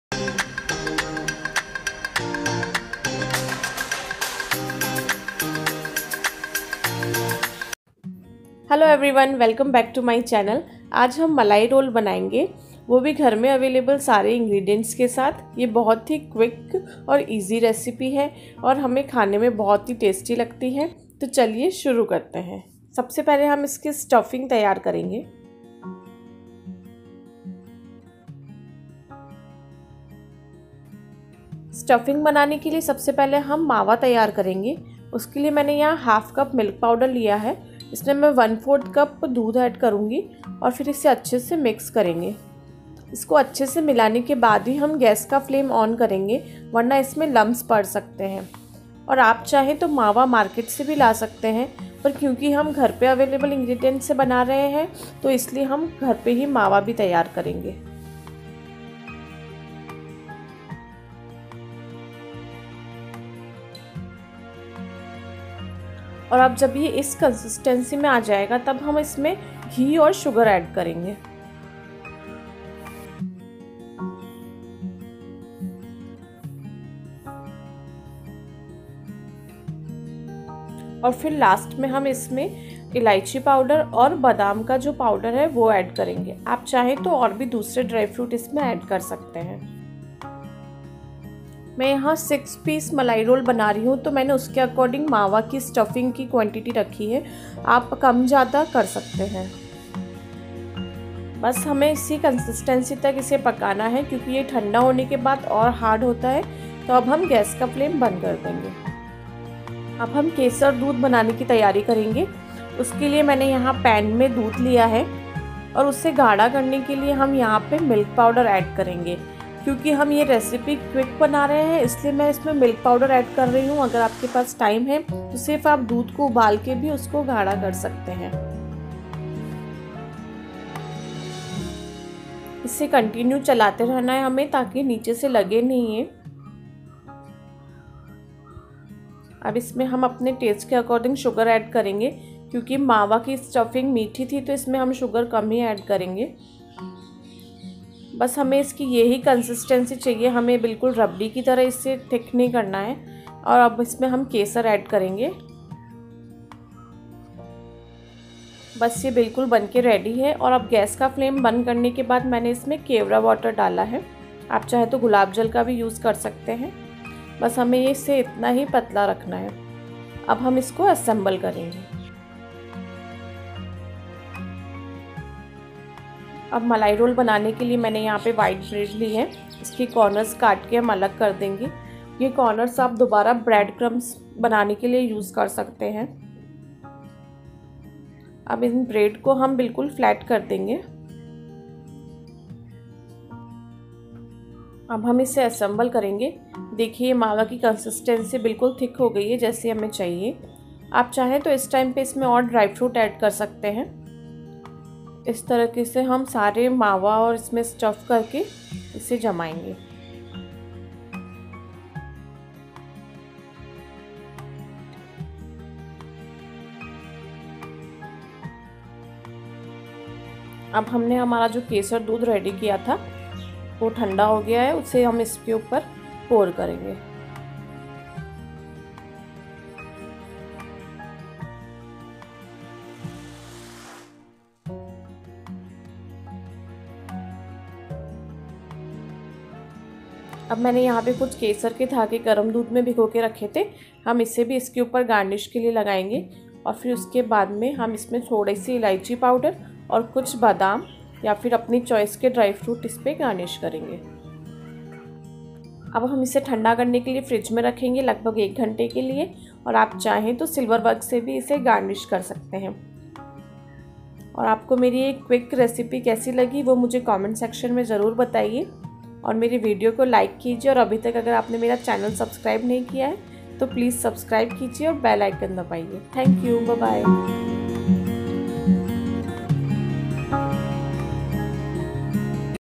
Bread Malai Roll | मलाई रोल | Quick Dessert
Bread Malai Roll

For Stuffing
Ghee
Sugar
Cardamom powder
Almond Powder
Kesar Milk
kevra water
sugar
also follow on Instagram
ketchup_chutny